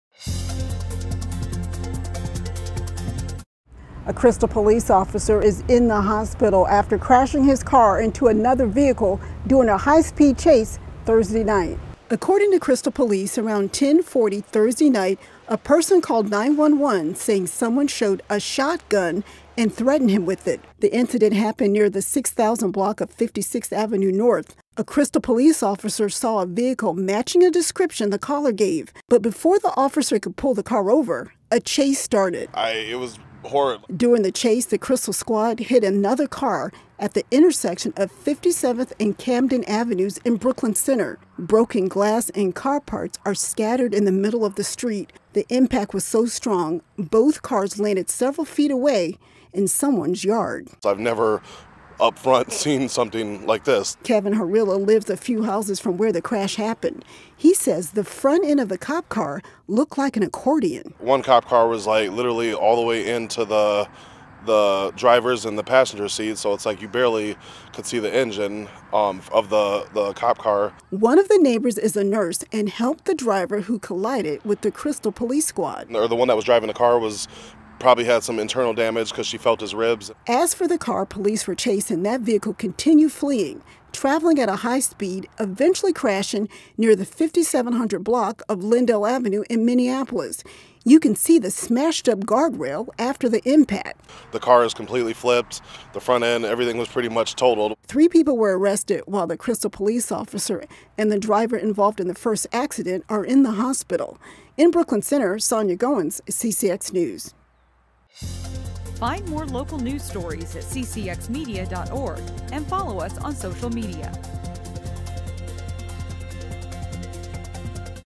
Crystal Police Officer Injured After High-Speed Chase
A Crystal police officer is recovering after being injured in a high-speed car chase. The officer was following a car that matched a report of a person threating another person with a gun.  The squad car collided with another car at the intersection of 57th and Camden Avenues in Brooklyn Center. Police arrested three people involved in the incident.